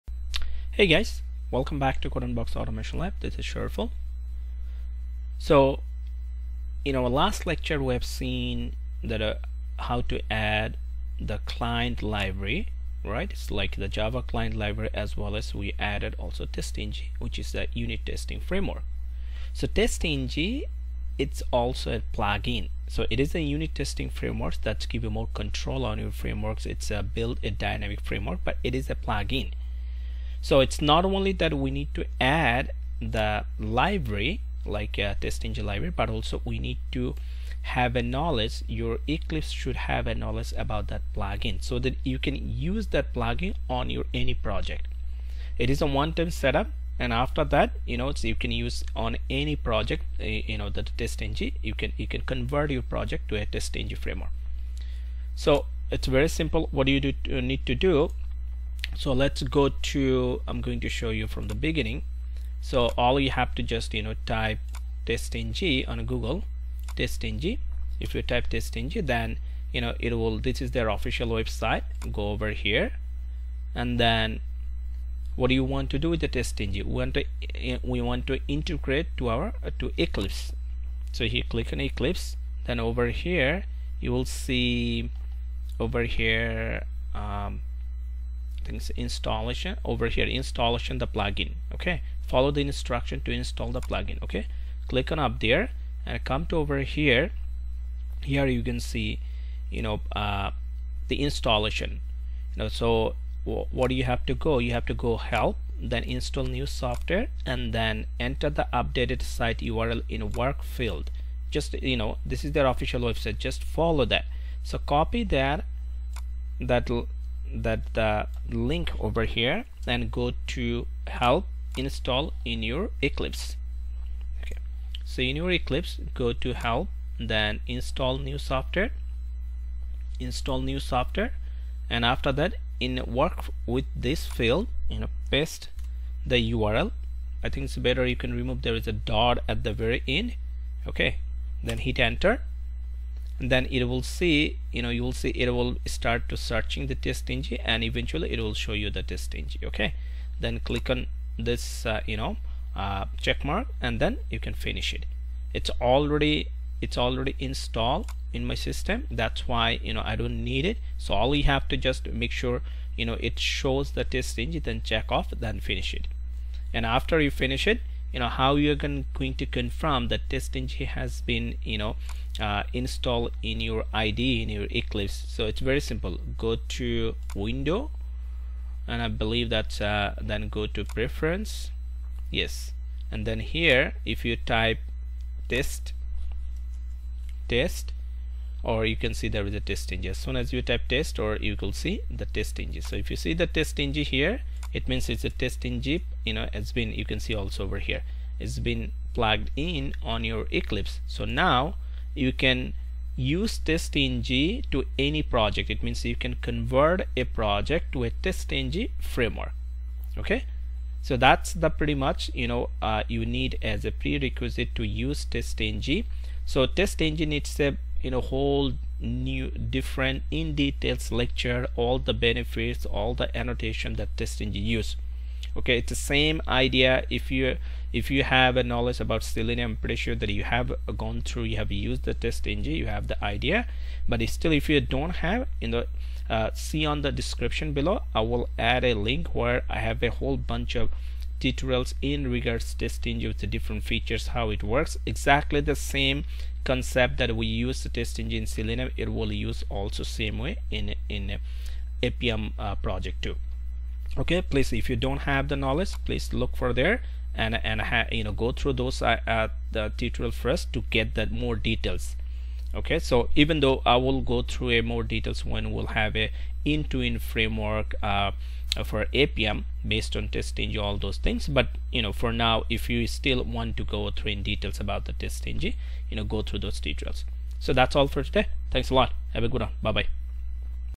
Appium Tutorial 5: How to Install TestNG plug-in to Eclipse for Appium automation
+How to Install TestNG plug-in to Eclipse?
+Integrate TestNG plug-in to Eclipse
▬▬▬▬▬▬  In this Video  ▶️  ▬▬▬▬▬▬
Install from update site
Select Help / Install New Software...
Enter the update site URL in "Work with:" field:
Make sure the check box next to URL is checked and click Next.
Eclipse will then guide you through the process.